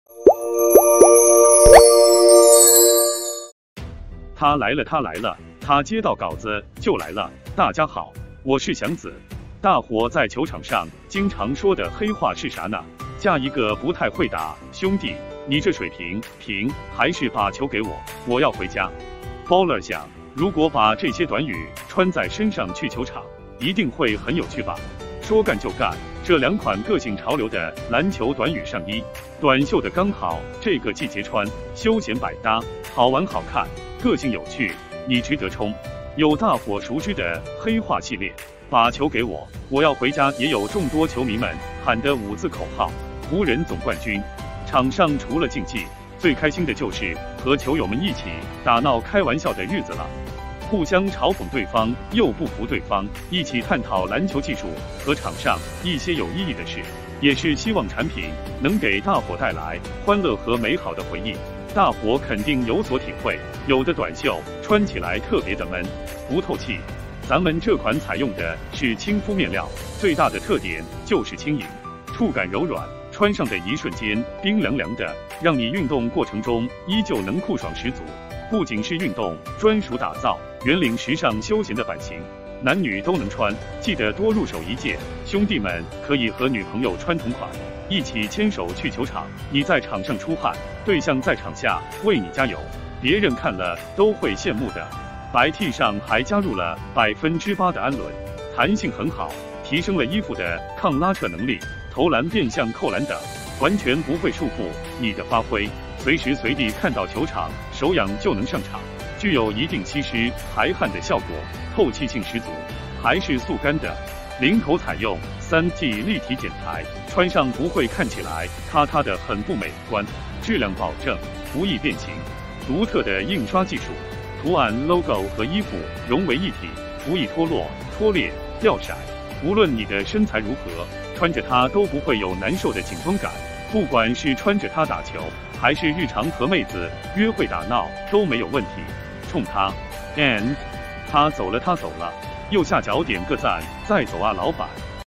穿得好，玩得开心！个性潮流的篮球标语夹克，和“湖人”冠军
大伙在球场上经常说的“黑话”是啥呢？“加一个不太会打”、“兄弟，你这水平（瓶）”还是“把球给我，我要回家”！baller想，如果把这些短语穿在身上去球场，一定会很有趣吧！说干就干，这两款个性潮流的篮球短语上衣，短袖的刚好这个季节穿，休闲百搭，好玩好看，个性有趣，你值得冲！